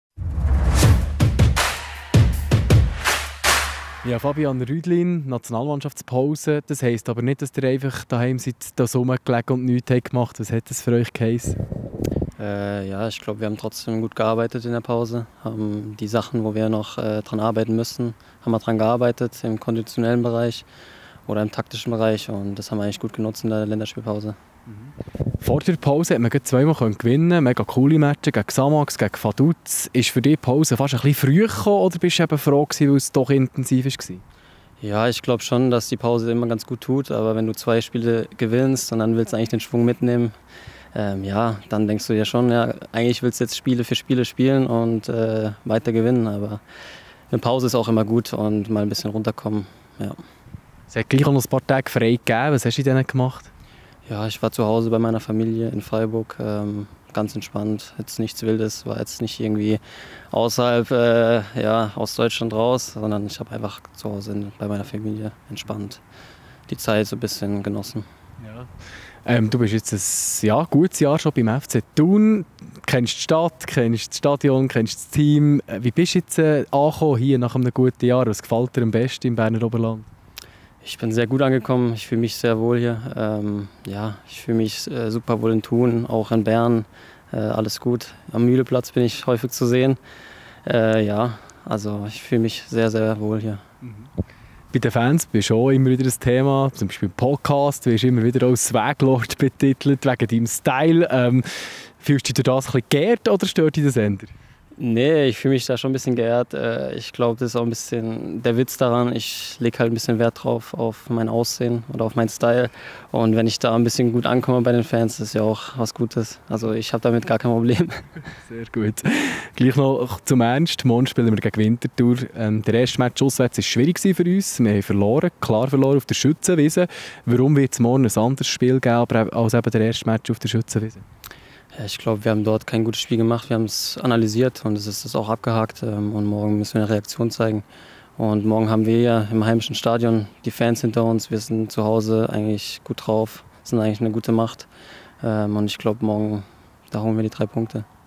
Interview mit Fabian Rüdlin
Der 24-jährige Mittelfeldspieler Fabian Rüdlin spielt seit gut einem Jahr beim FC Thun Berner Oberland. Was dem gebürtigen Freiburger in Thun am besten gefällt und was er dazu sagt, dass ihn gewisse Fans mit dem Spitznamen «Swag Lord» betiteln, seht ihr im Video.